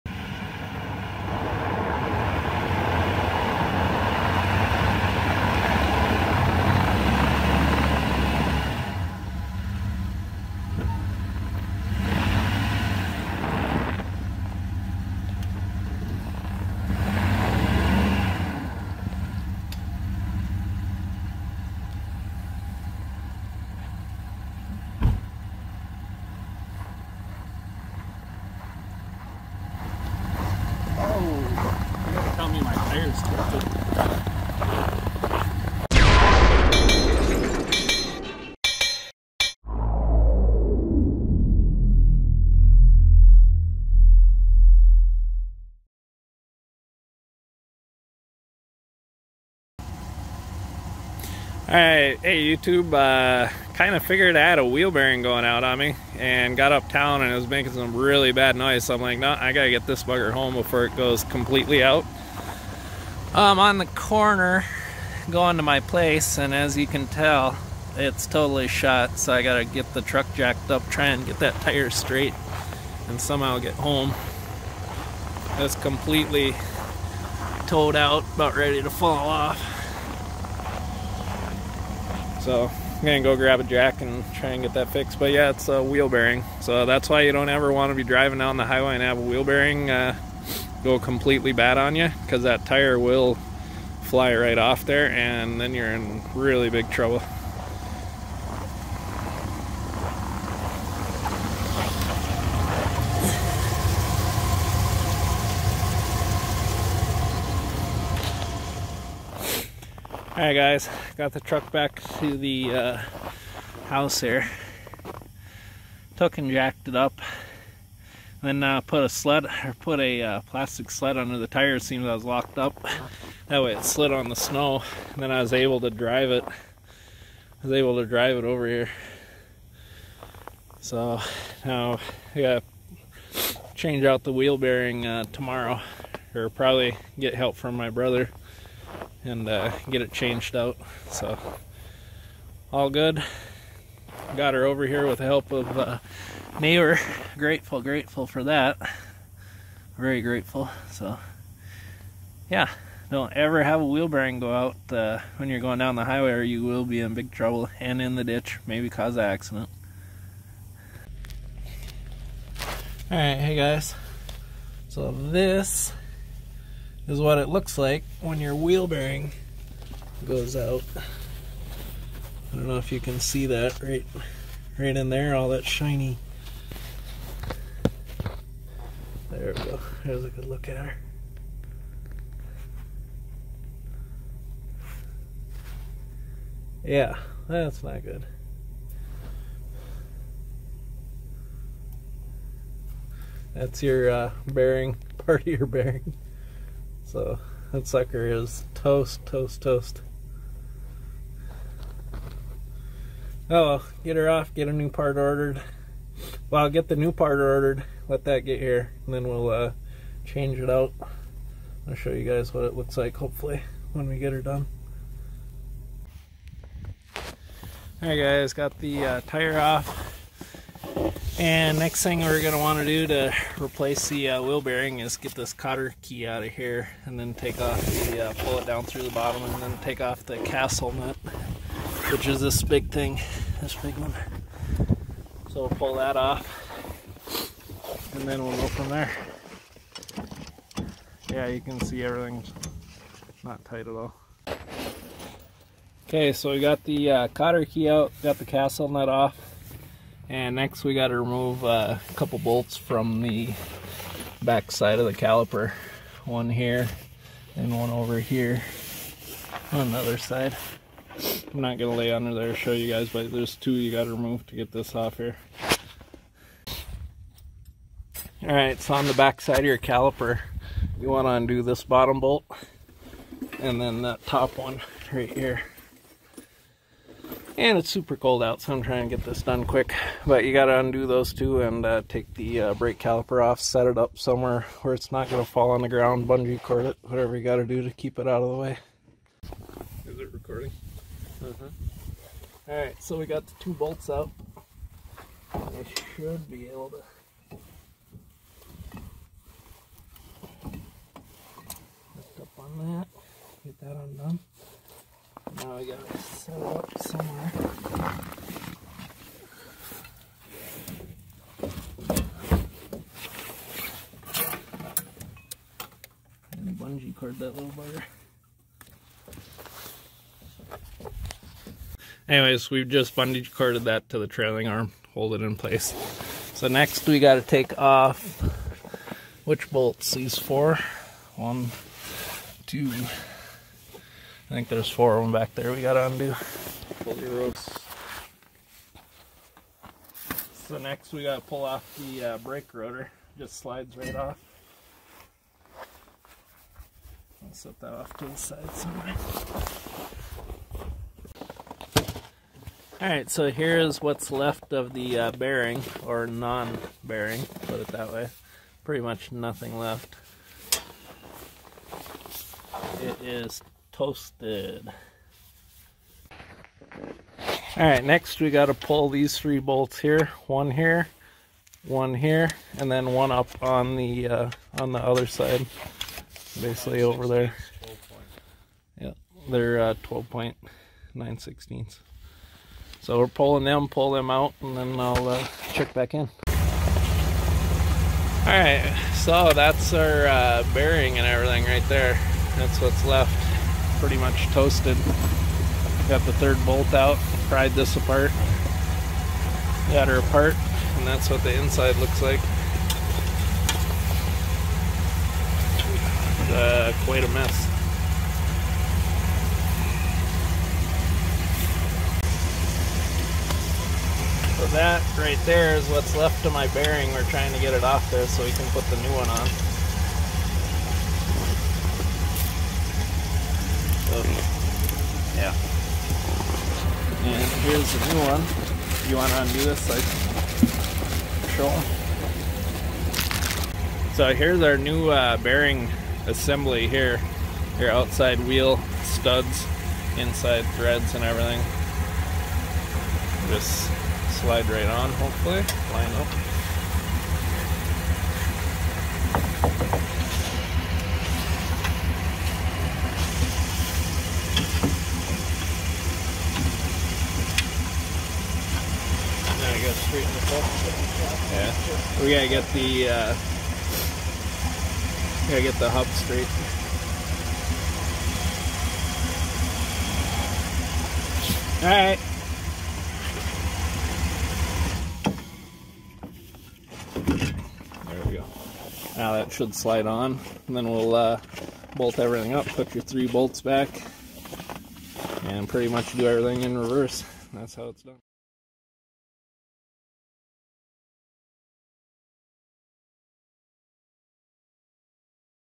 My Tire Fell off! How to replace a wheel bearing
So after quite a while of hearing a high pitch squeak coming from my front right passenger side and multiple trips to the locals shops where I told them about what I was hearing and that i probably have a wheel bearing going out to which they kept checking and saying they didn't see anything. My wheel bearing finally went out all the way! Thankfully I knew what was happening so I turned around to go for home very slowly hoping we would make it there. You NEVER want to be driving full speed down the highway with a wheel bearing going out or your tire could fall all the way off and you could be seriously hurt or killed in an accident as a result.
     So on the last stretch before we were home the tire locked up completely and the truck was having a hard time pushing the tire on the slippery snow, which is where this video starts. I had not hopped out of the truck yet and didn't know the tire was tilted and ready to fall off!.. With the help of a car jack and a neighbors plastic sled we were able to lift up the front and get the tire to slide on the sled enough to get the truck home to make repairs there.